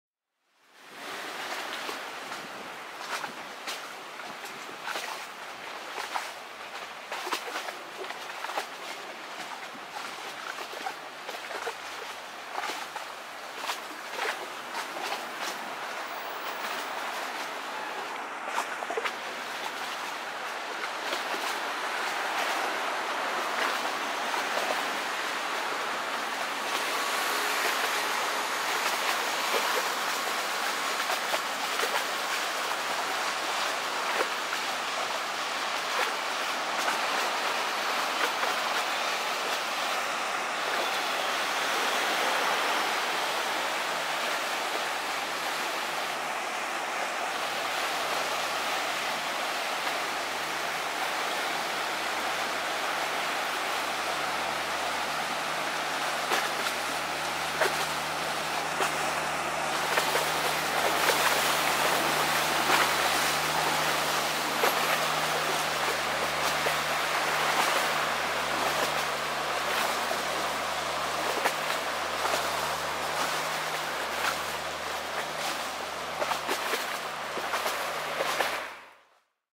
⛴ 'RELINQUENDA' vrachtschip, 03290298, gespot 13 09 2019
RELINQUENDA, 800 tons vrachtschip met een 550 pk Volvo Penta diesel, één van de dingen die ooit weer verlaten moeten worden, zoals de naam zegt..